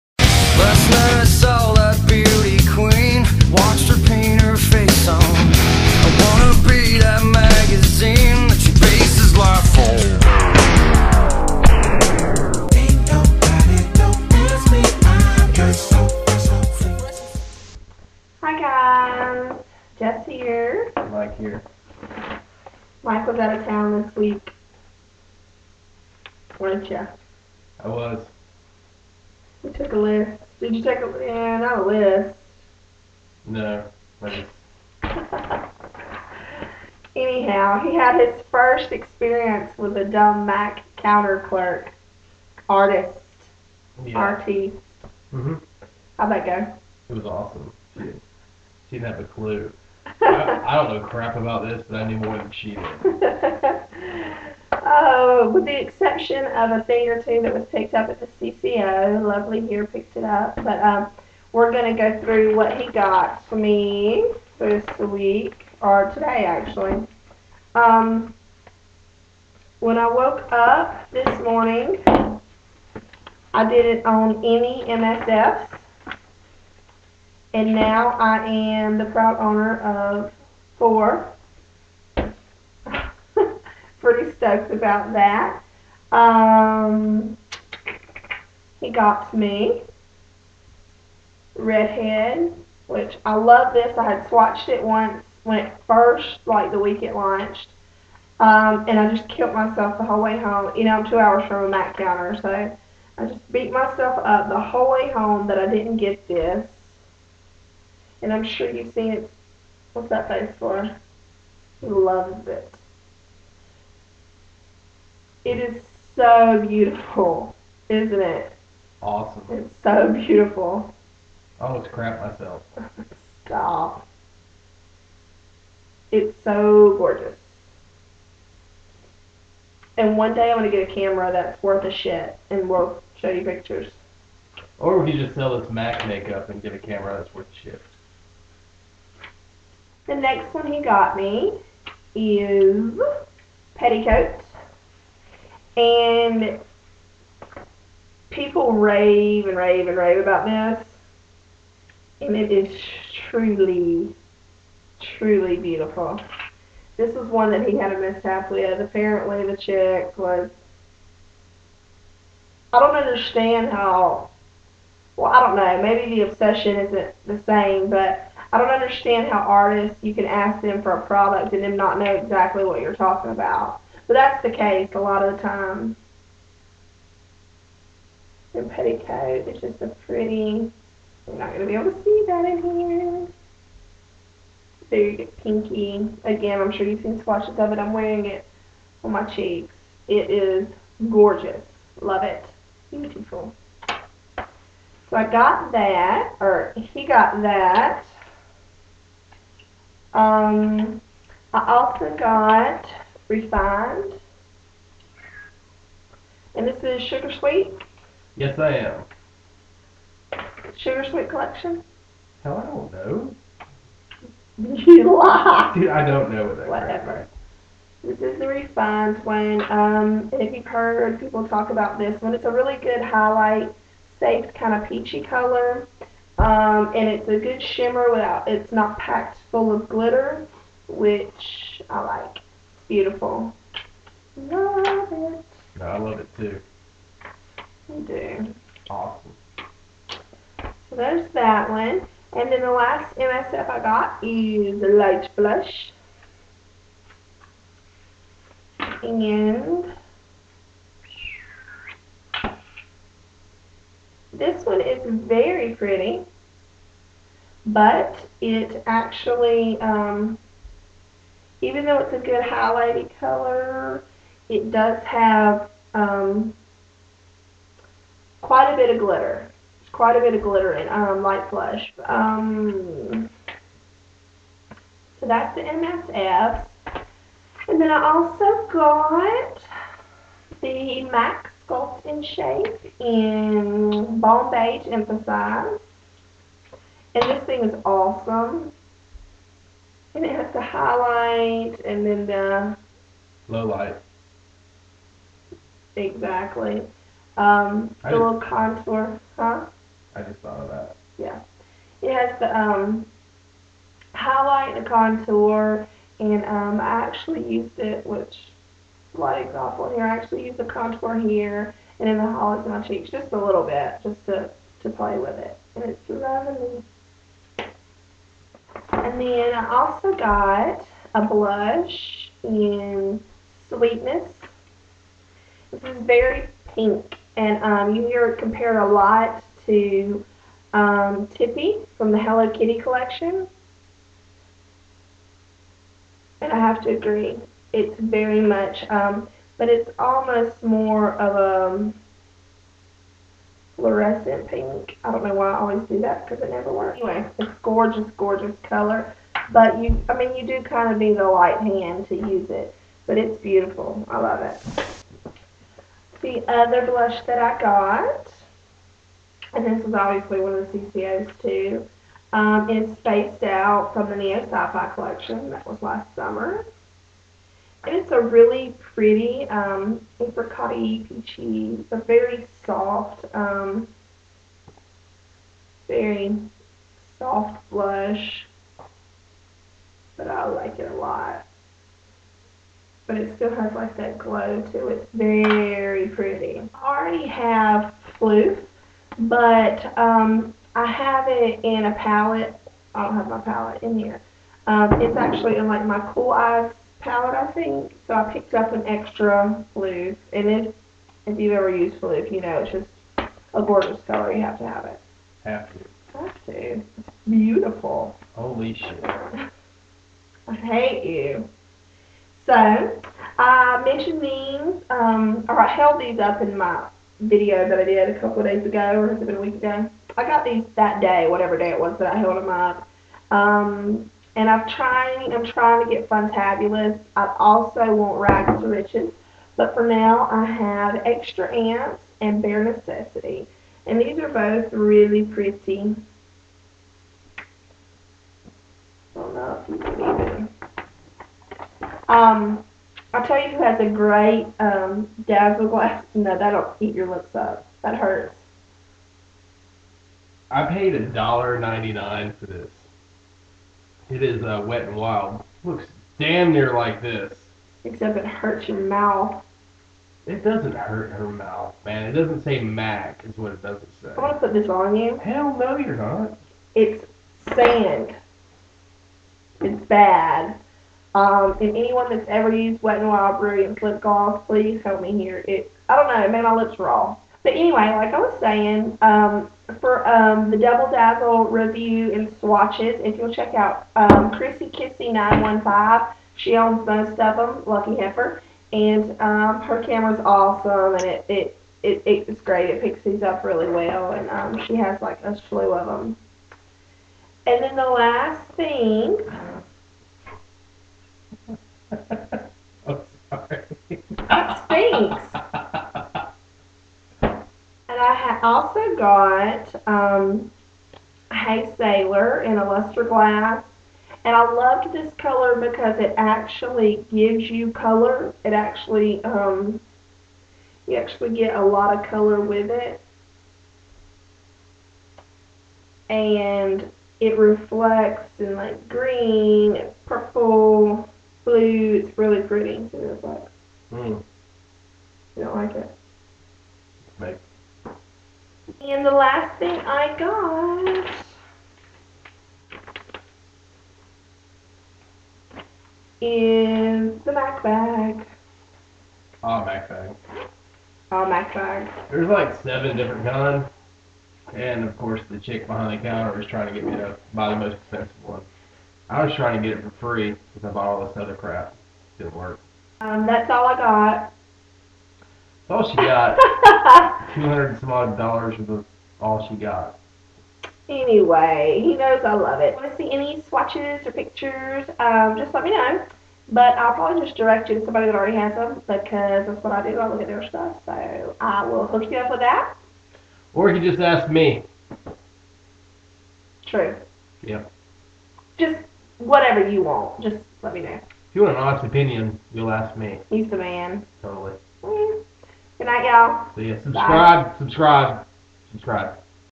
The Hubby Haul
The honey fixed me up! Just sharing the new goodies